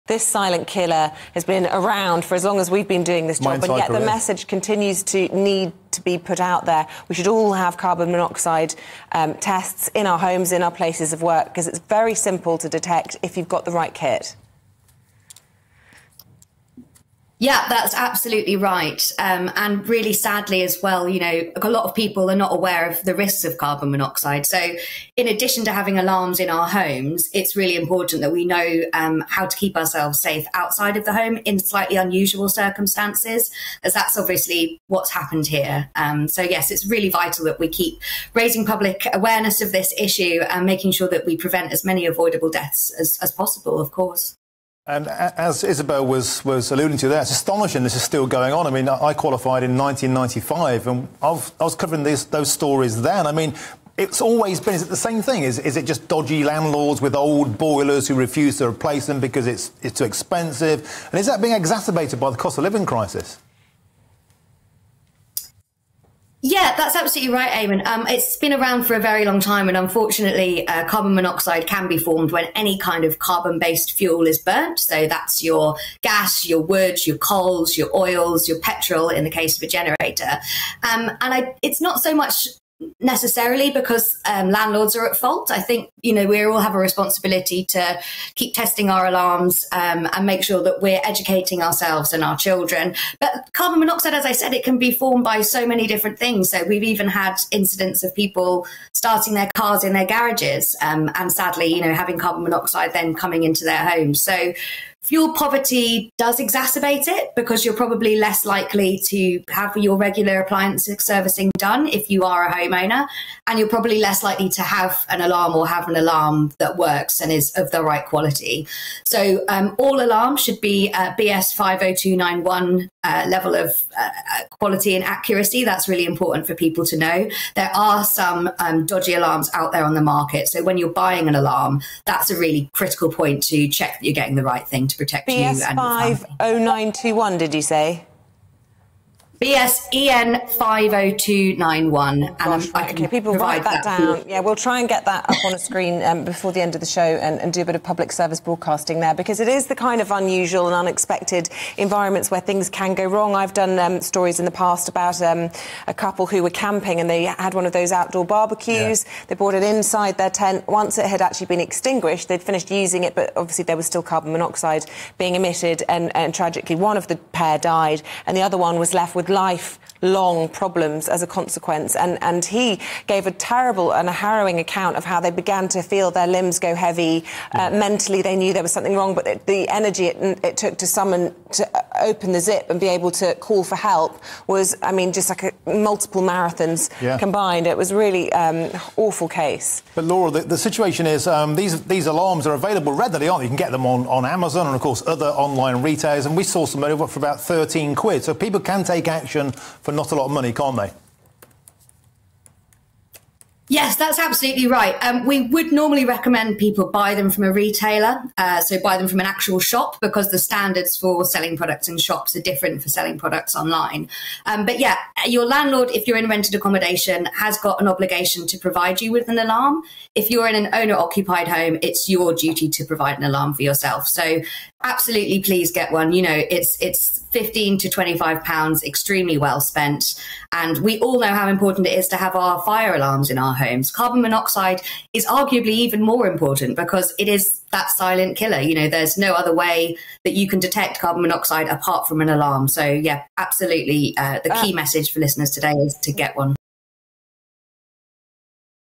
Mother and daughter found dead in van on valentine's day from carbon monoxide poisoning
'It's really important we know how to keep ourselves safe from carbon monoxide, especially in unusual circumstances, that's likely what's happened here.'

Laura Fatah from Carbon Monoxide Group, reacts to the death of a mother and daughter in Kent from carbon monoxide poisoning.